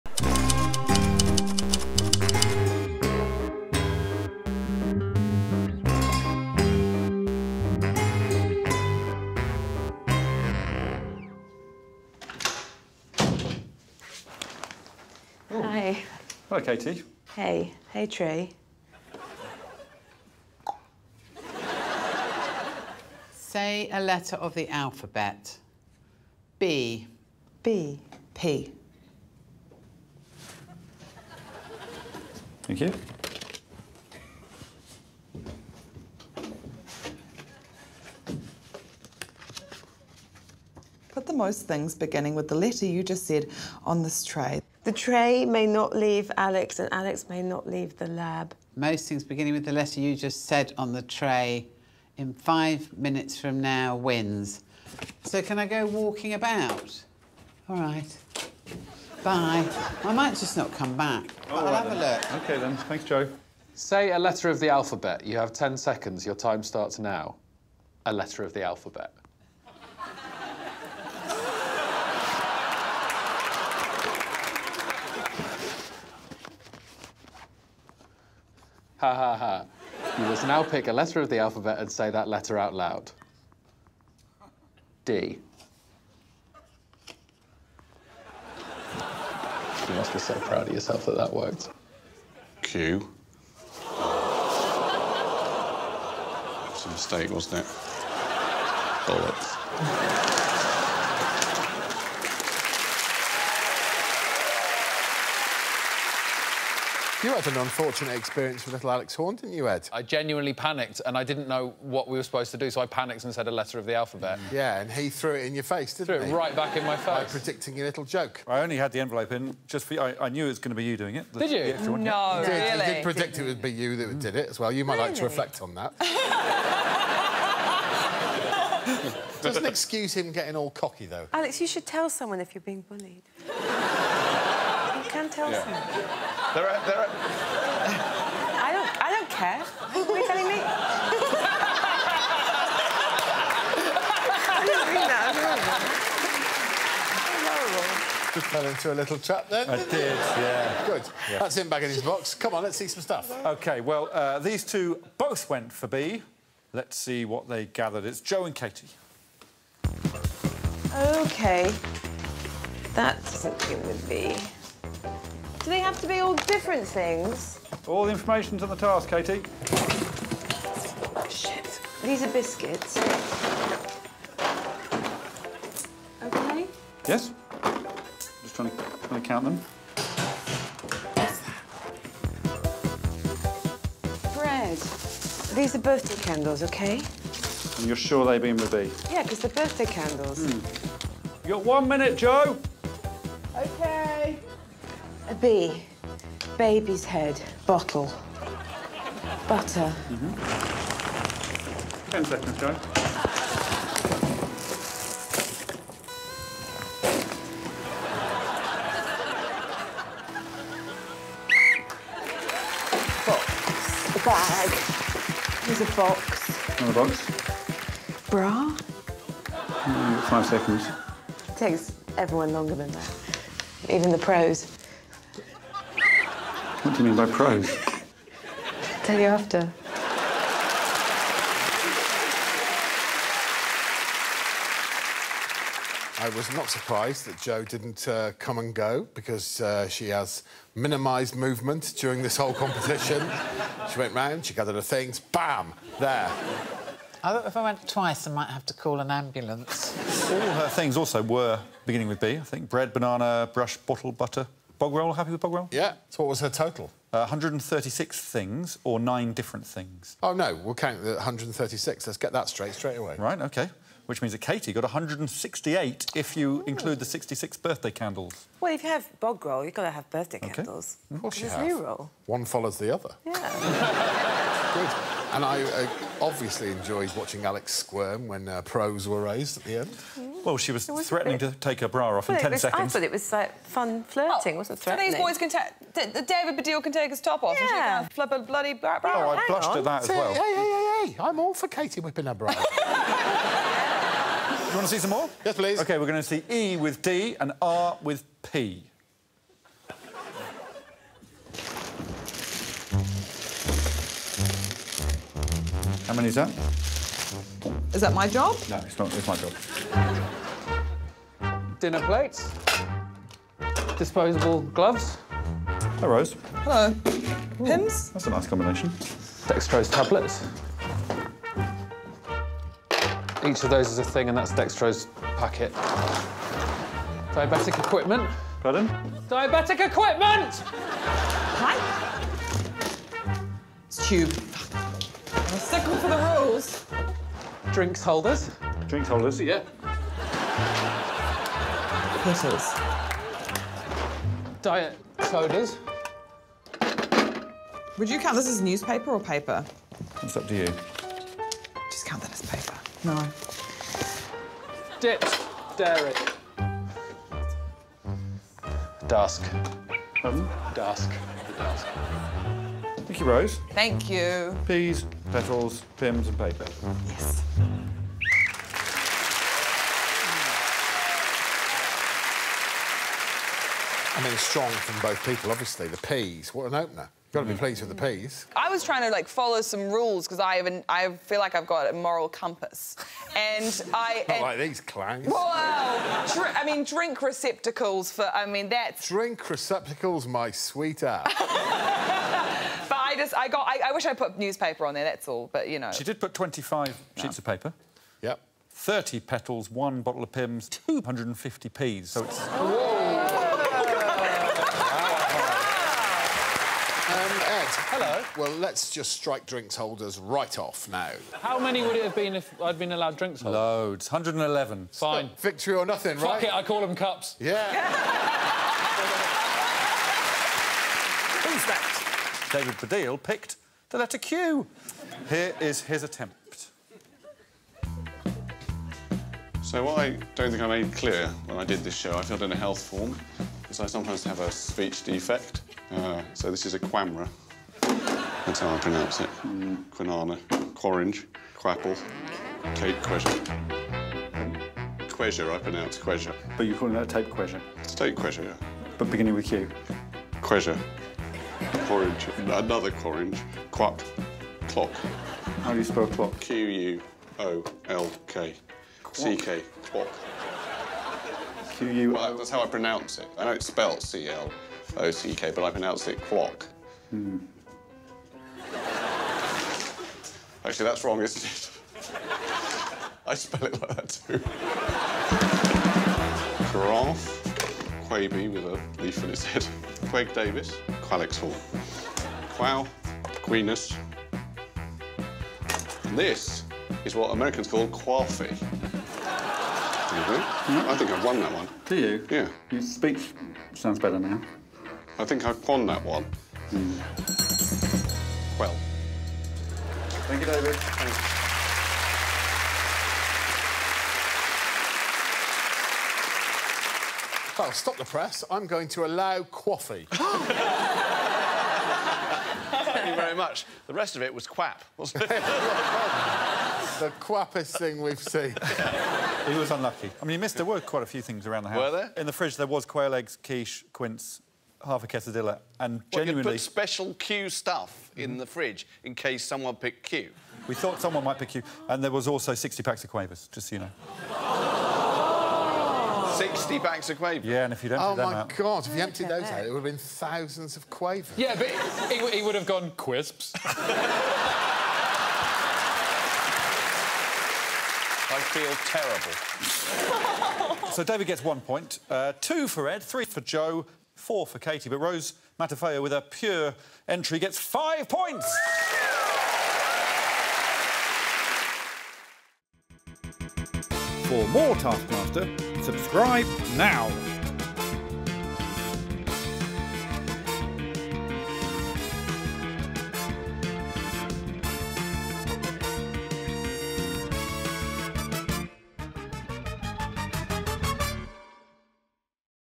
Put the Most Things Starting with 'B' on the Tray | Full Task
Put the most things beginning with your letter of the alphabet onto the tray.